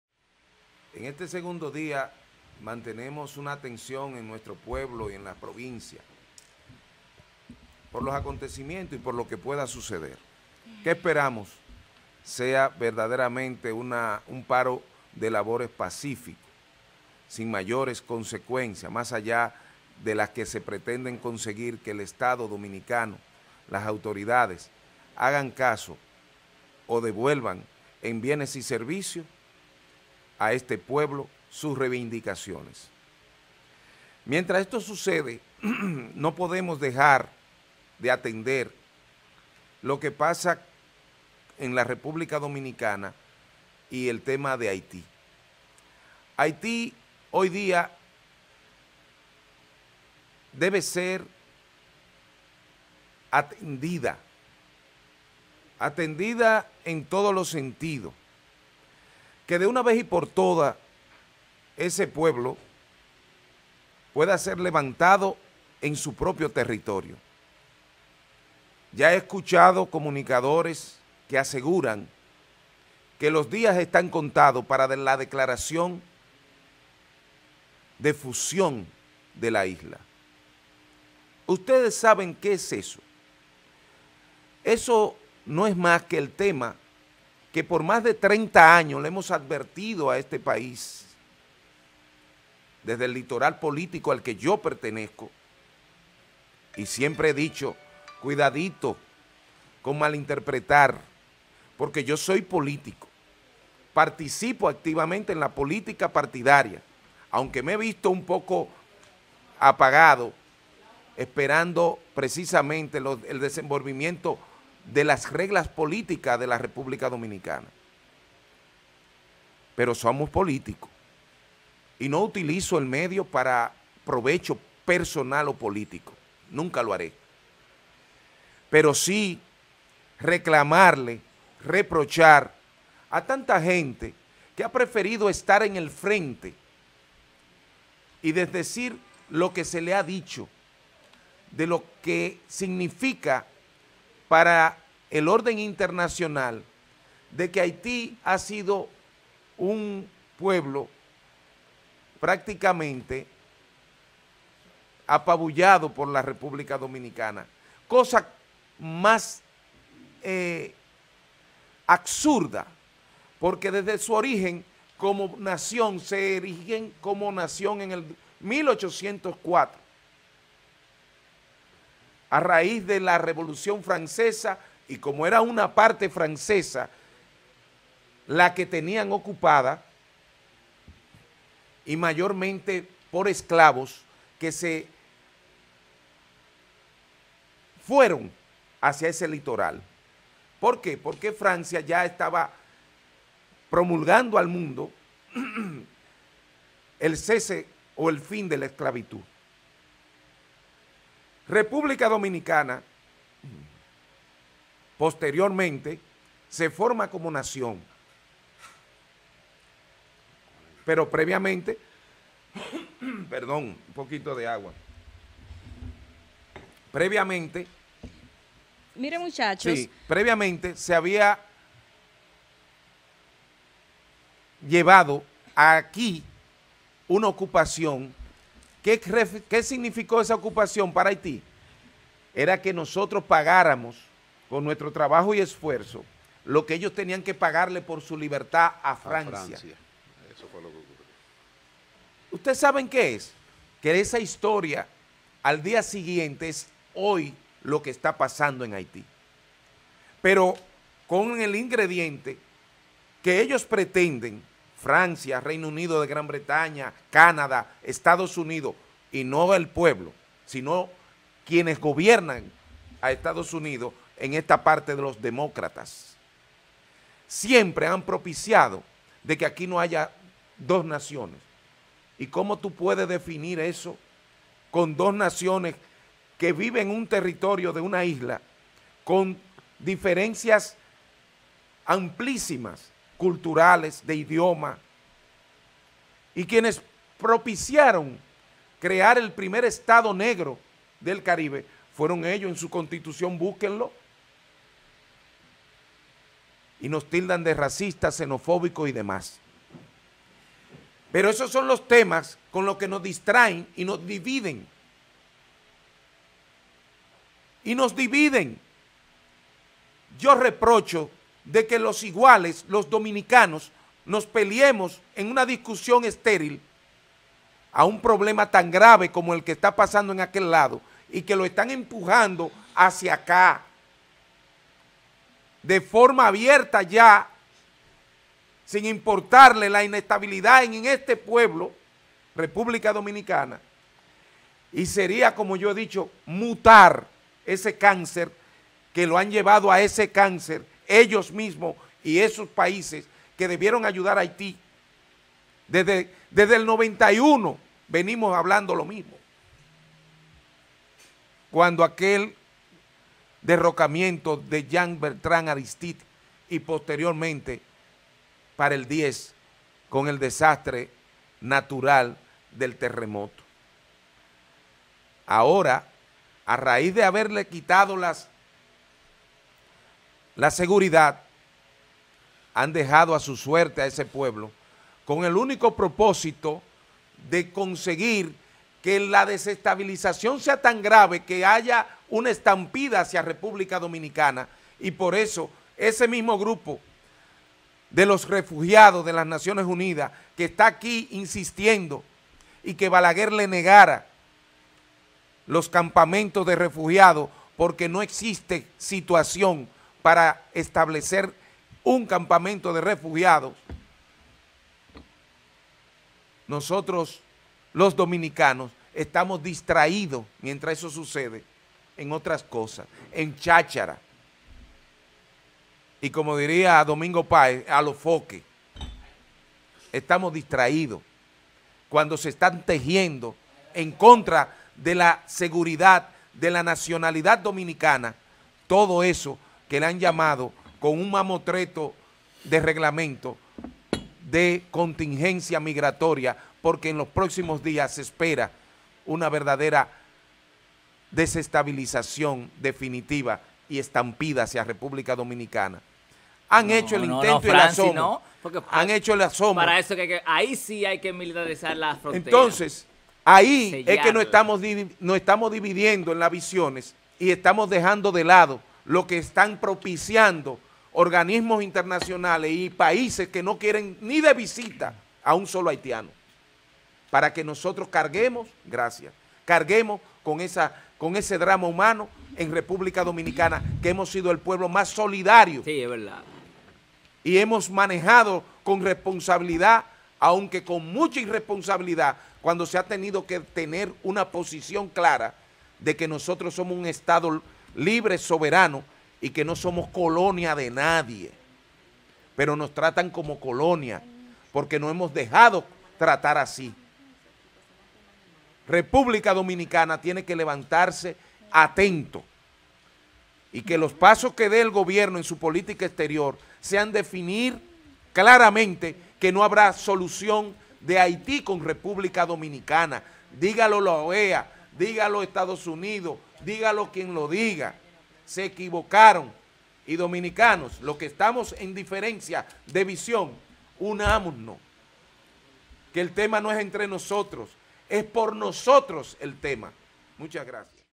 No dejemos que nos distraigan de la gravedad de Haití
No dejemos que nos distraigan de la gravedad de Haití sumida en inseguridad, falta de gobernabilidad,  las pandillas, hambre y en salubridad